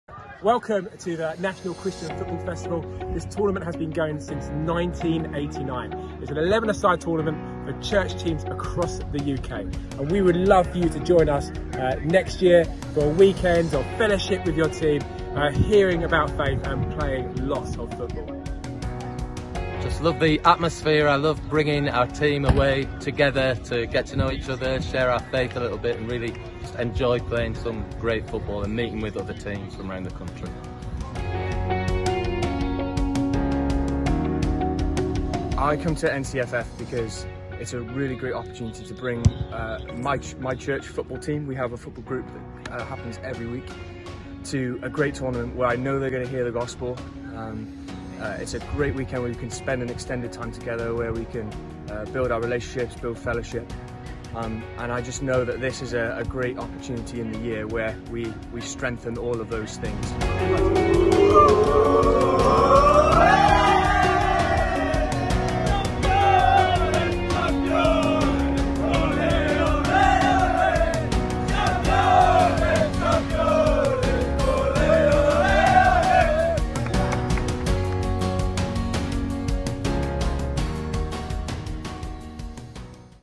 National Christian Football Festival 2024
Promotional video for the 2024 NCFF tournament which is taking place on Saturday 13th & Sunday 14th July 2024 at Harper Adams University.